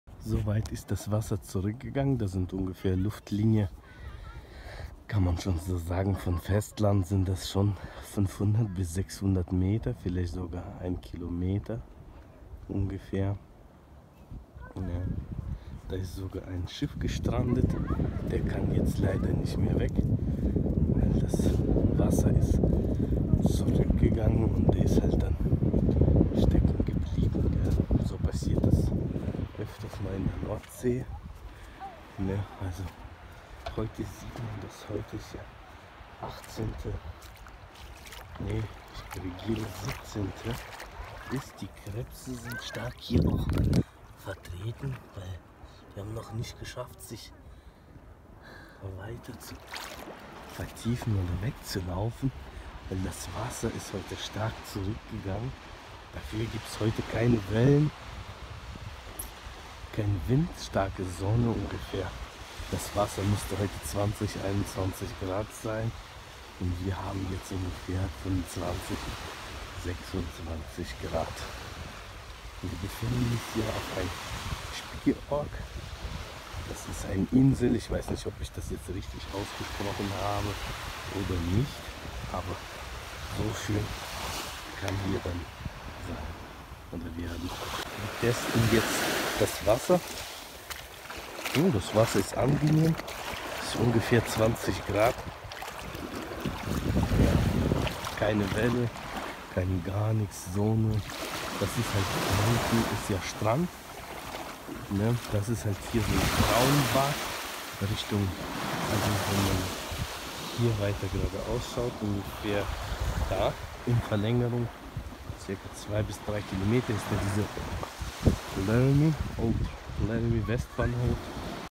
Nordsee, Germany. Insel Spiekeroog. Urlaub in Deutschland 🇩🇪.
Liebe Grüße, endlich hab ich meine Videos sortiert und kann jetzt bearbeiten. Ich hoffe ich kann es gut da stellen. Ich möchte gern auch euch alles zeigen, dass auch bei uns in Deutschland schön sein kann. Freunde die Insel ist relativ klein aber man kann was erleben. Друзья у нас в Германии очень красиво добро пожаловать в гости.